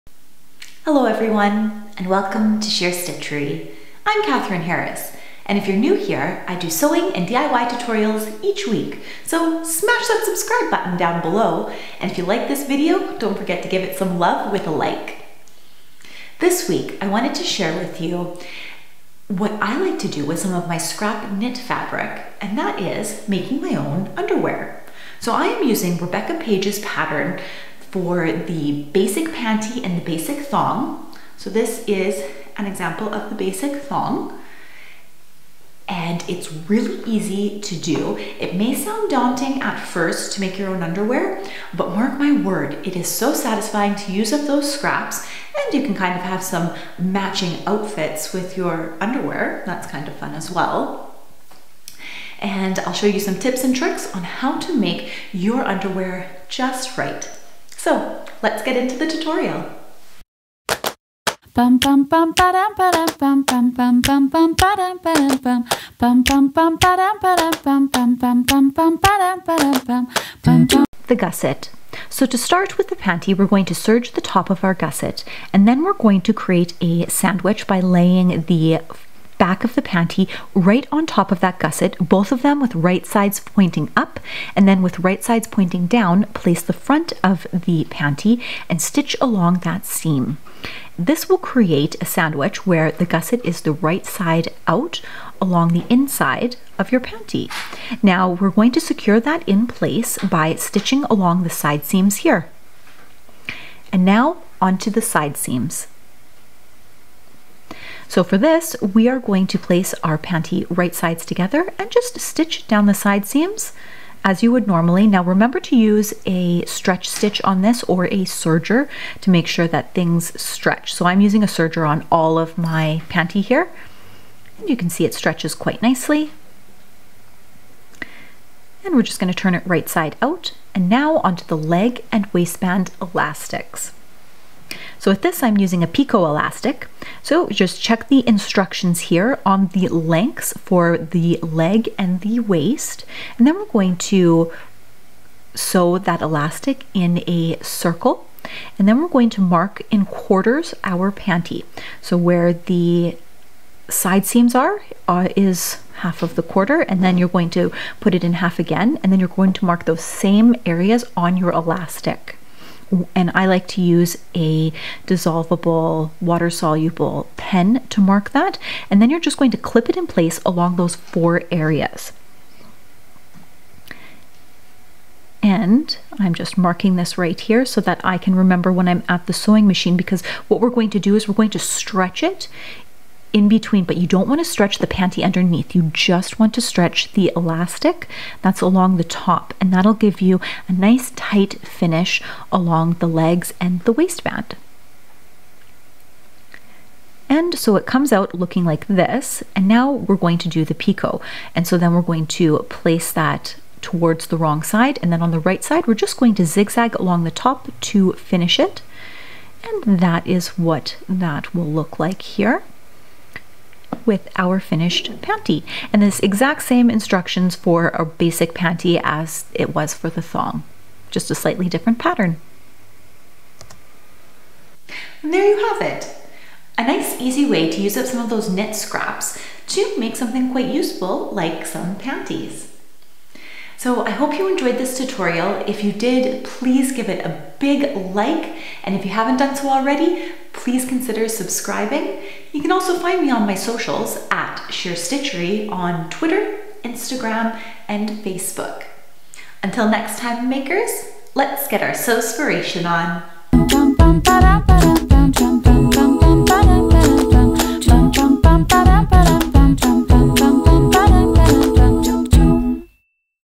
DIY Panties - How to Sew Panties
Hello Makers and welcome to Sheer Stitchery!

In this sewing video I share how to use up knit scraps by sewing them into panties and thongs.

This is a a really fast make and they come together nicely so there is no reason to put off sewing panties or making a pair of DIY panties.

⭐️TIMESTAMPS⭐️
00:00 - Intro
00:27 - Cutting with Chiffon
03:33 - Marking Your Fabrics
04:51 - Sewing with Chiffon
07:41 - Stitching the Seams
08:37 - No Backstitching
09:44 - Finishing the Seams
11:24 - Hemming Chiffon
 13:51 - Outro

- Fabric:  Recycled from old tees

Get tons of sewing freebies and updates on new pattern releases.

As always, if you have any questions, please leave them in the comments down below.

Until next time Makers and Creators let's get our 'Sew-Spiration' on!